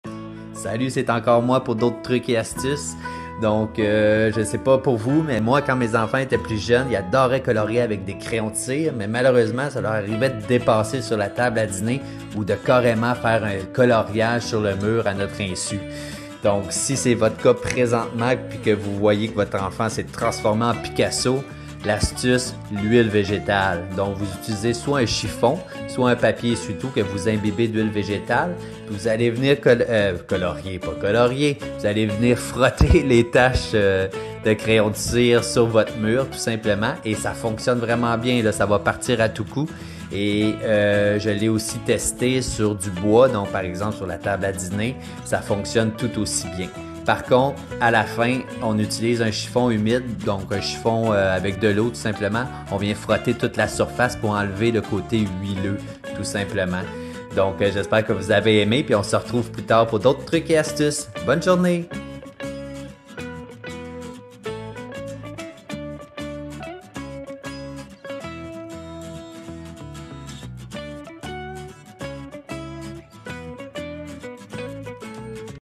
Truc pour enlever des taches de crayon de cire sur un mur blanc
Votre enfant a fait un coloriage sur votre beau mur blanc ? Pas de panique, vous pouvez enlever facilement et naturellement les marques de crayon de cire sur un mur grâce à l'huile végétale.

D'autres trucs et astuces pour enlever les marques de crayon cire sur les murs juste ici :